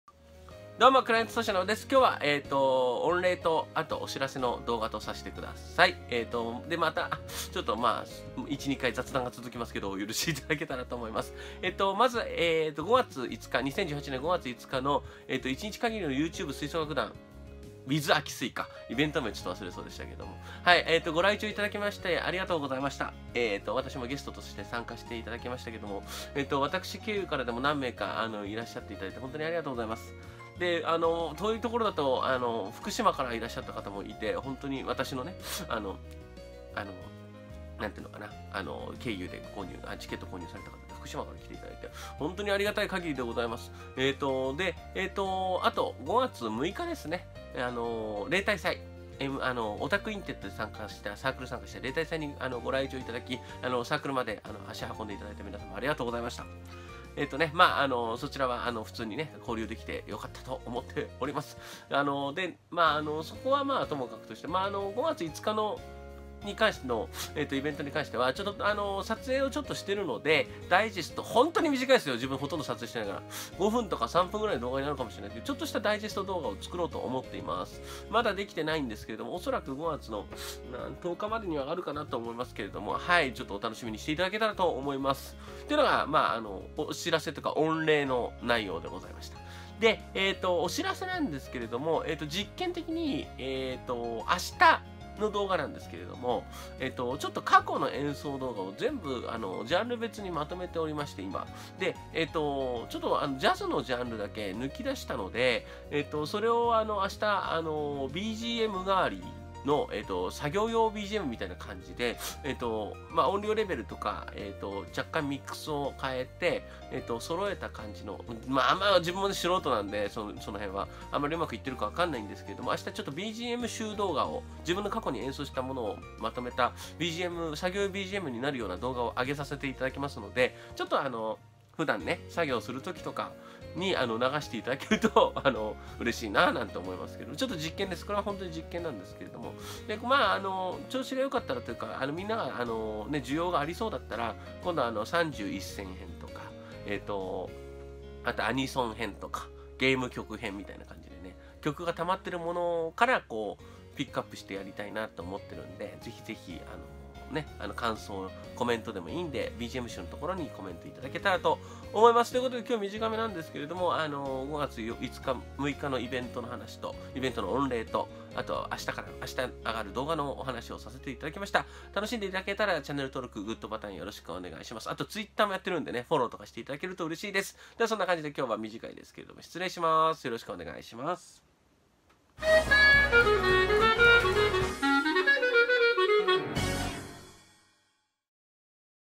【クラリネット雑談】イベントの御礼と明日の動画のお知らせ
クラリネット奏者ナオです！今日は御礼とお知らせの動画です！！

他の演奏参加チャンネル
■おたクインテット